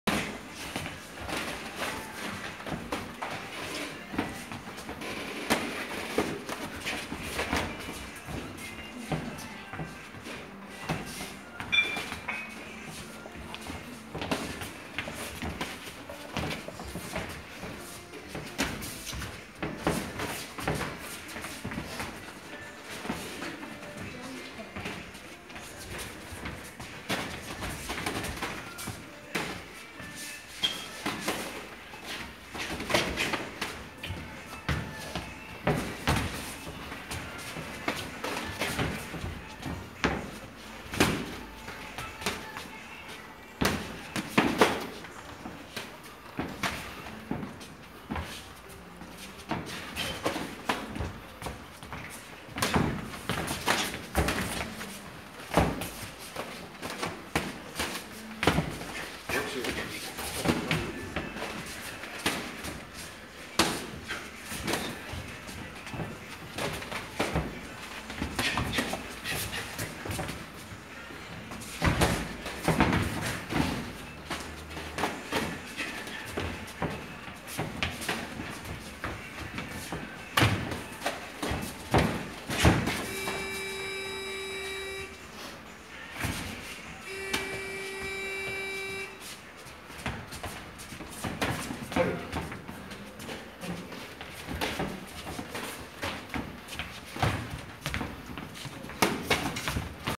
대전 서구 내동 홍수환 복싱 클럽
매스복싱
규영(블랙)
프로세빈(화이트)

대전 서구 내동 홍수환복싱클럽
세계챔피언 배출관장님 직접 지도